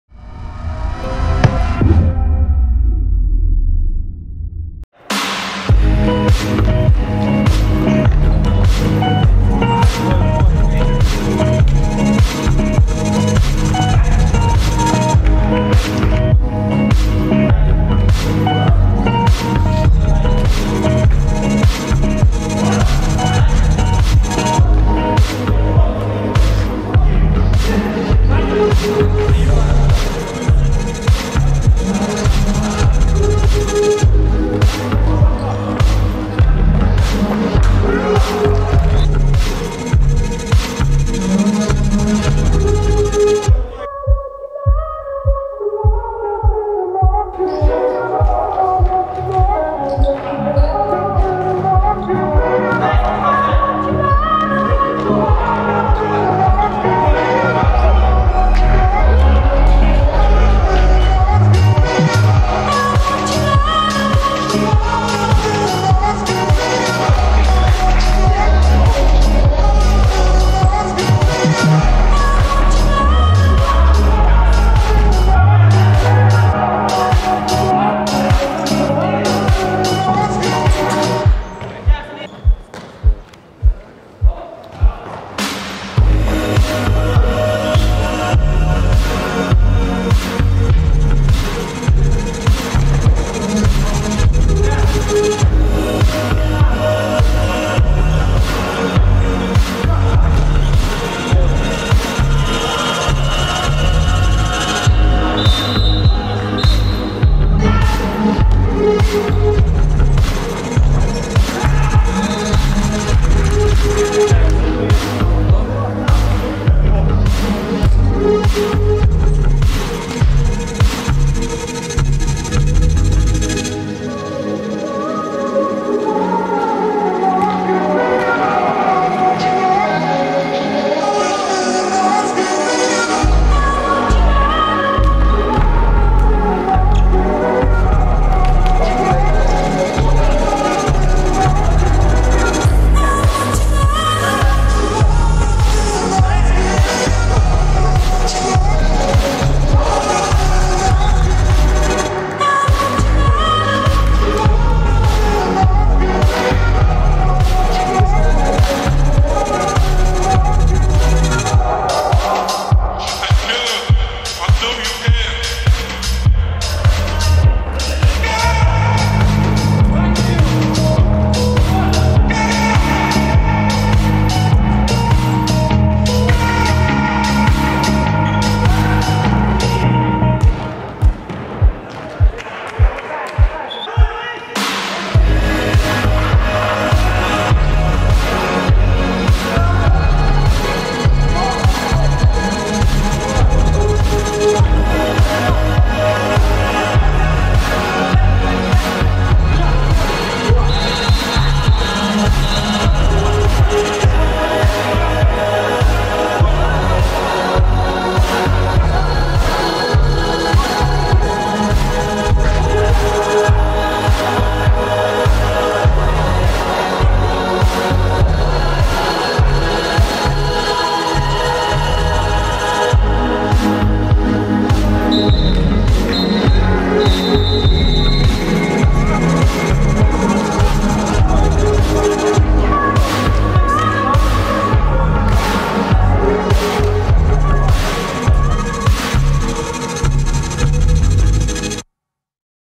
Спільнота - Галіо [Огляд матчу] (Перша ліга. 10 тур)
Перша ліга футзалу Львова, сезон 2015/2016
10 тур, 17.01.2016
Спільнота - Галіо 2:2